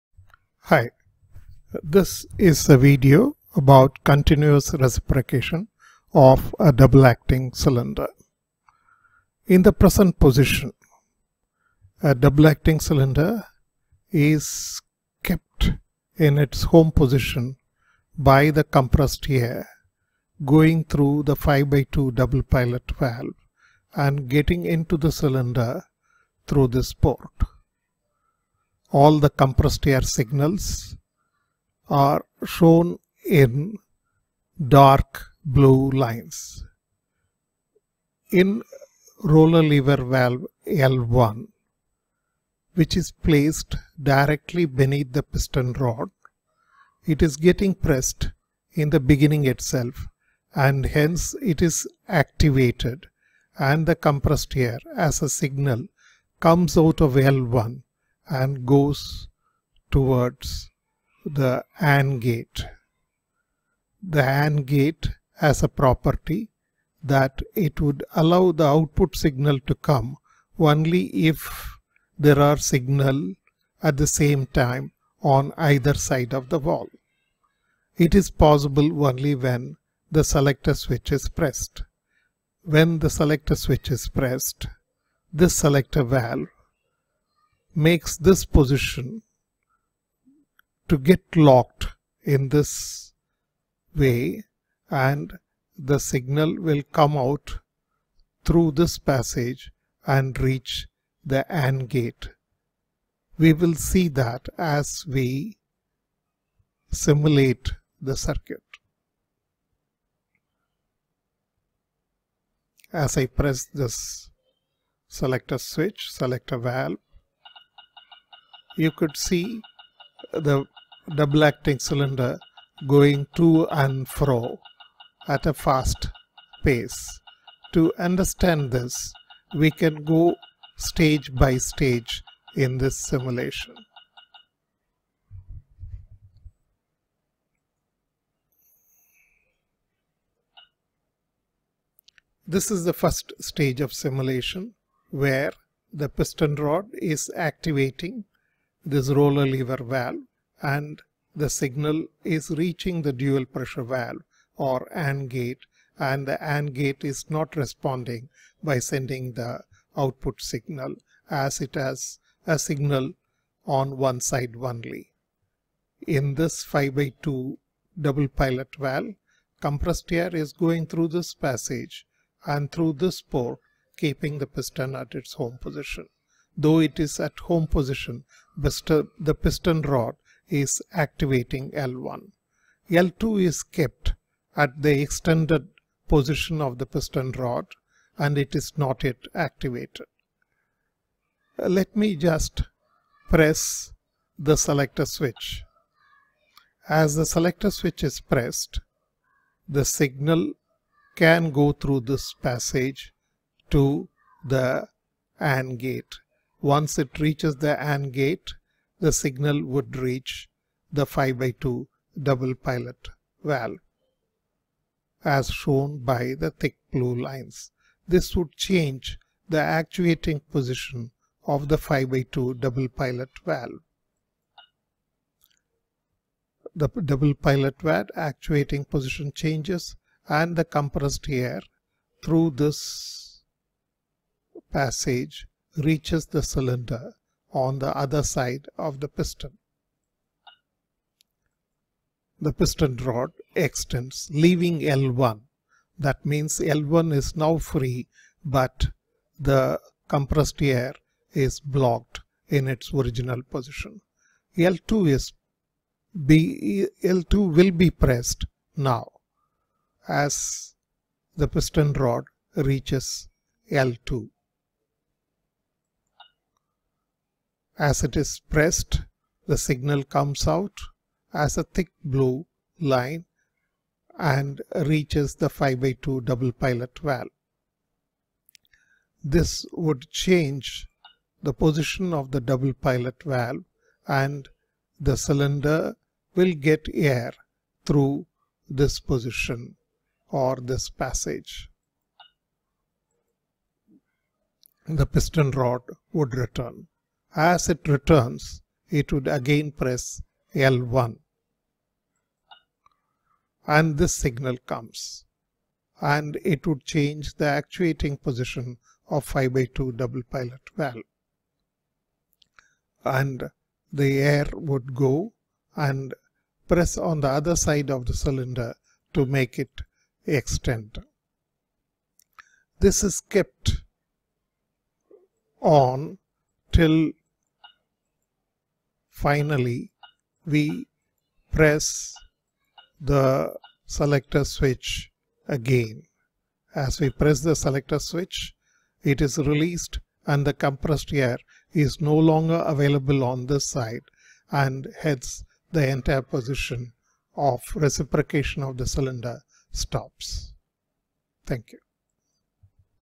Pneumatics & Pneumatic circuits
Explains the continuous reciprocation of double acting cylinder